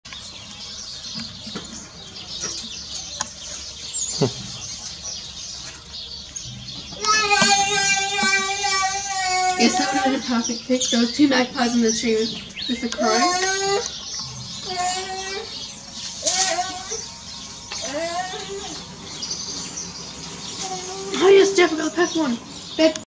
Nikon P900 83x Optical and 166x Digital world record zoom of bird in a tree - World record 1080p HD
Zooming in on a Rosella in a tree